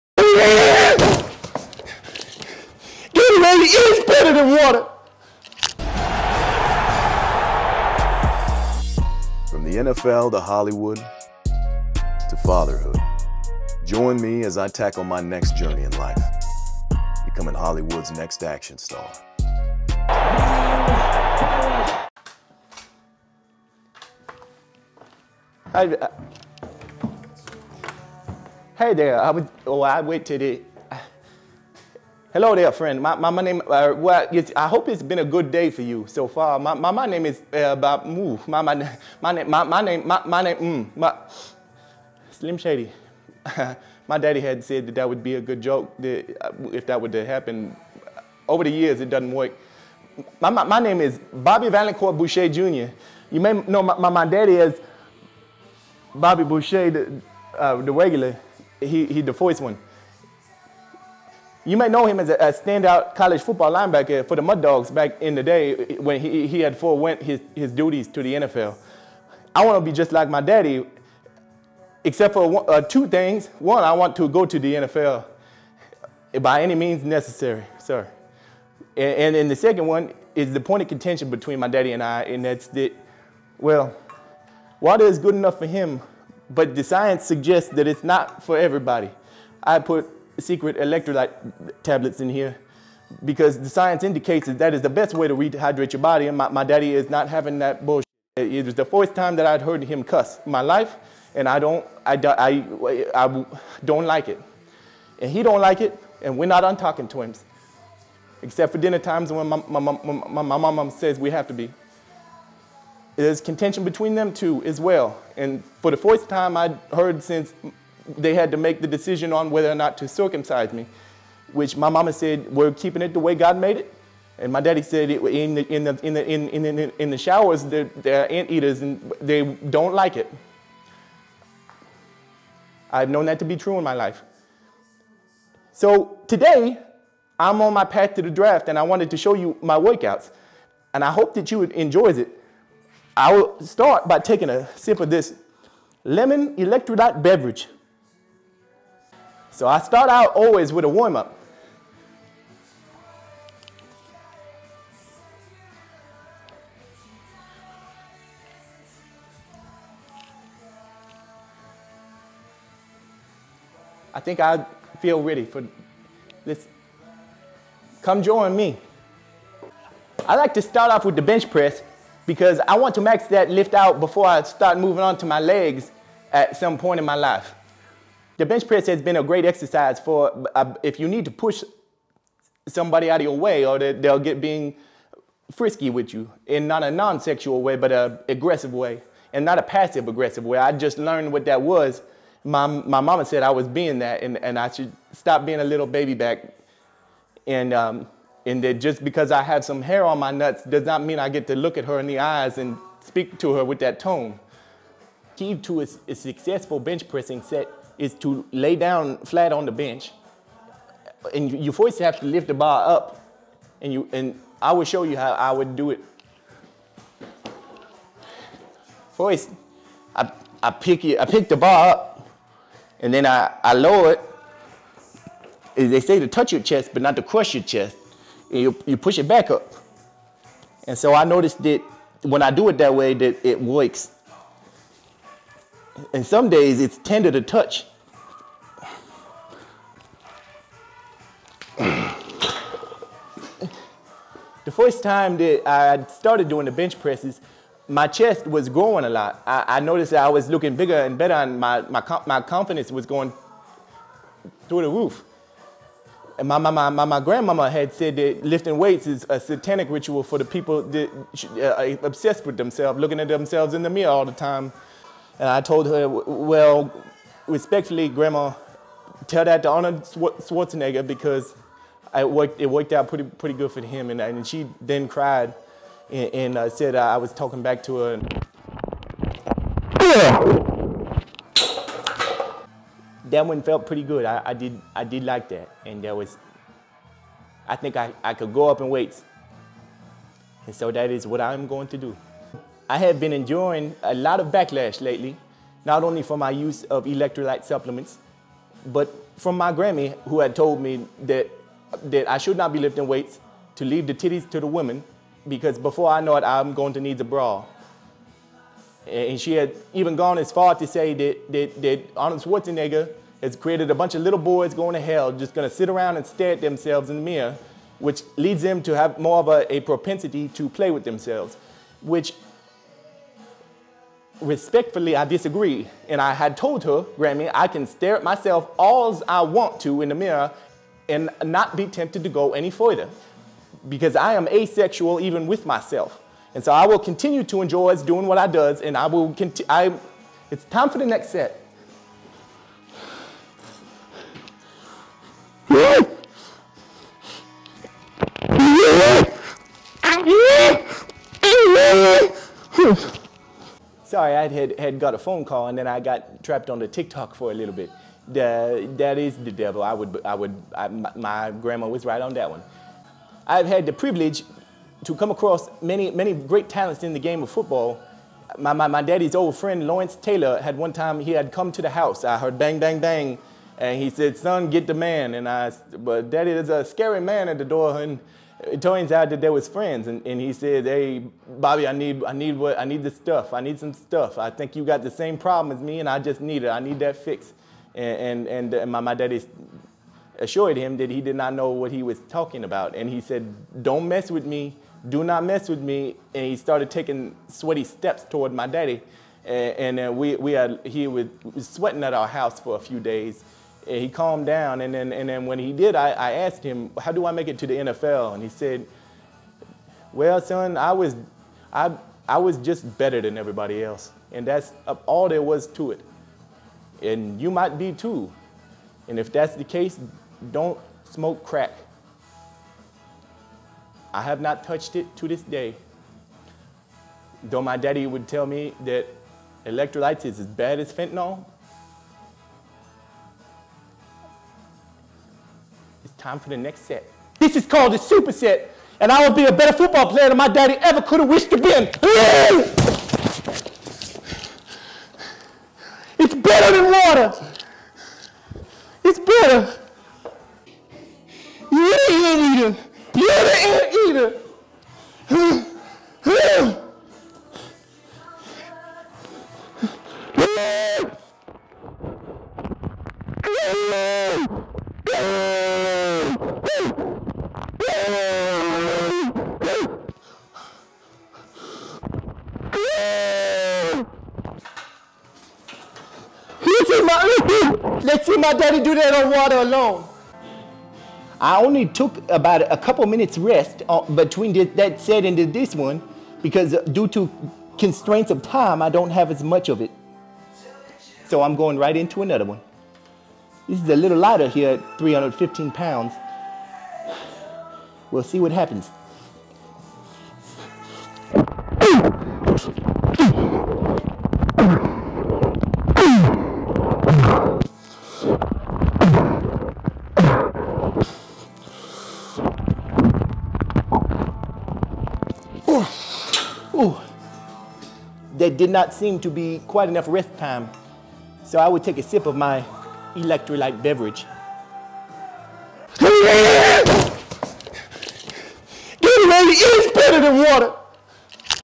The Waterboy’s Son is NFL Bound | Becoming Ep 51
Join Bobby Boucher's son, Bobby Vallencourt-Boucher Jr.,  on his path to the NFL draft. In this video, he discusses the challenges of tradition vs. science in hydration, the influence of his legendary father, and his own fitness and football aspirations. Bobby's story is filled with humor, determination, and a touch of rebellion. Dive into his workout philosophies, family dynamics, and the humorous yet insightful anecdotes that define his path to self-improvement.

🌟 Miles Burris: From NFL Linebacker to Hollywood Action Star.

Chapters:
0:27 - Hydration Hacks: Why Bobby believes in science over tradition when it comes to staying hydrated.
0:51 - Family Ties: The influence of Bobby's father, the Mud Dogs legend, and their differing views on water.
1:22 - The Electrolyte Debate: How Bobby's preference for electrolyte-enhanced water creates household friction.
2:01 - Workout Wisdom: Dive into Bobby's workout regimen, starting with the bench press and his goals for physical improvement.
2:58 - Confronting Criticism: Bobby addresses backlash from his traditionalist grandmother and defends his fitness journey.
4:42 - Celebrity Encounters and NFL Aspirations: A story from Bobby's past about meeting a drug induced football hall of famer and getting candid advice.
6:03 - The Super Set: Witness Bobby's intense workout routine designed to prove he's more than just his father's son.
7:55 - Electrolyte Victory: Bobby's triumphant moment showcasing the benefits of his hydration strategy.